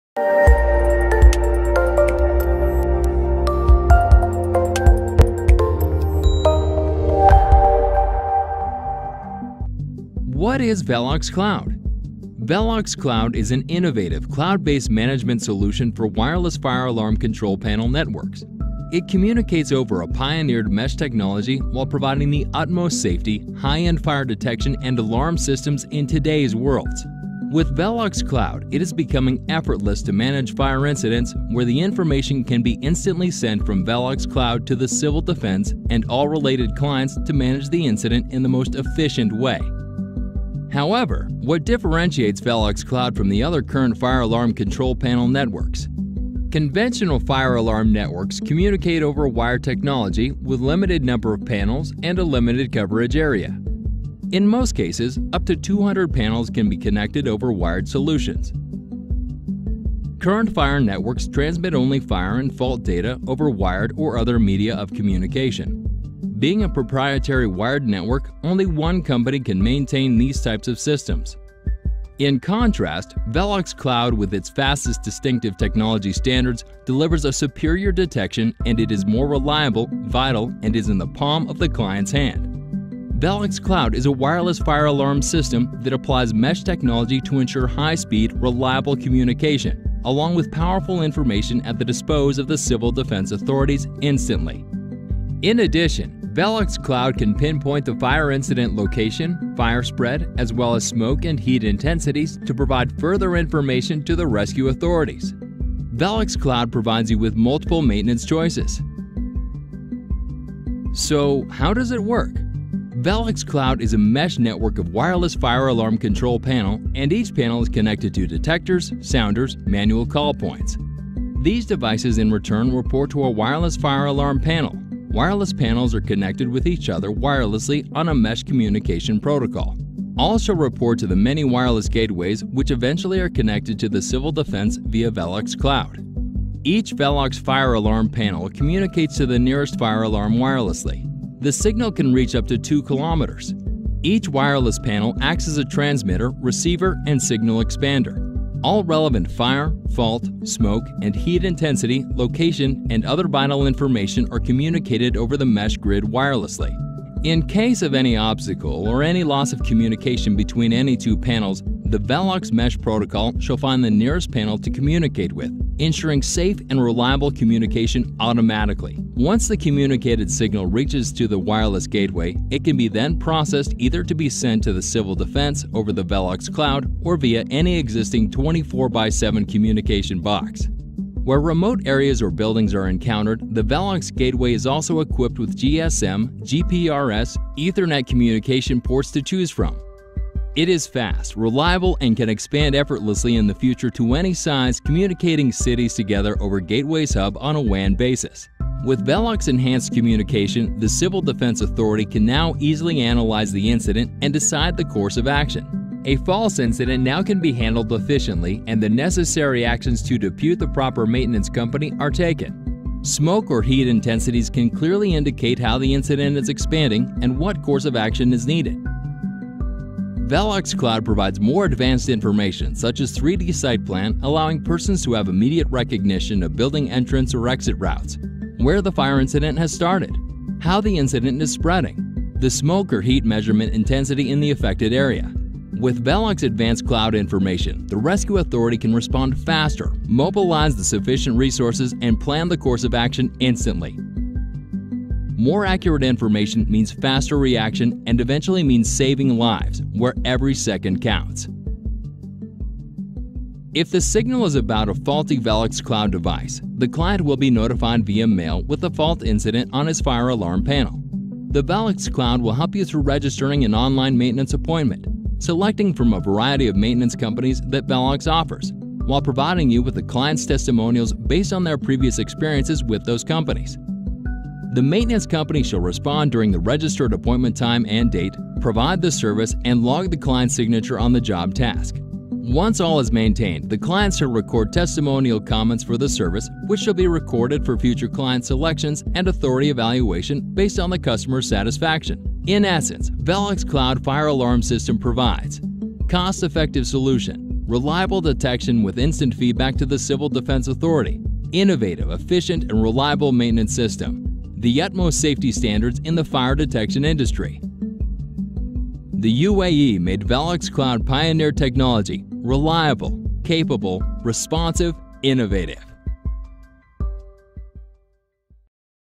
VELOX Cloud - Smart Fire Alarm Solution
Today VELOX CLOUD is not just a concept; it is already a reality. We are linking wireless mesh fire alarm panels over a citywide application, and allowing the control from the palm of the user.

The VELOX CLOUD is equipped with AI "Artificial Intelligence" making the service of fire or fault identifications easy tasks to handle. We believe it is simply the future of fire detection and alarm systems.

To conclude of what VELOX CLOUD can do:

•   Smart city connectivity over 868MHz IOT. "Internet of Things."

•   Integration capability with Smart Street Lighting as one CLOUD Service.

•   Fully monitored GUI "Graphical User Interface" in the palm of your hand.

•   Monitors smoke and heat intensity via the VELOX smoke detectors connected to the VELOX TOUCH fire alarm panel.

•   Pinpoints the fire incident location and fire spread

•   Comprehensive maintenance over any smart device or PC, with all records, logged and archived on the VELOX CLOUD.

•   Over 10,000 VELOX TOUCH panels can be connected to a single gateway.

•   Each VELOX CLOUD gateway can communicate over GSM, LTE, GPRS, or simply Ethernet.

ATEIS Middle East is proud that the VELOX CLOUD is ready to ship today!

ATEÏS Middle East FZCO.
LIU No.11 Dubai Silicon Oasis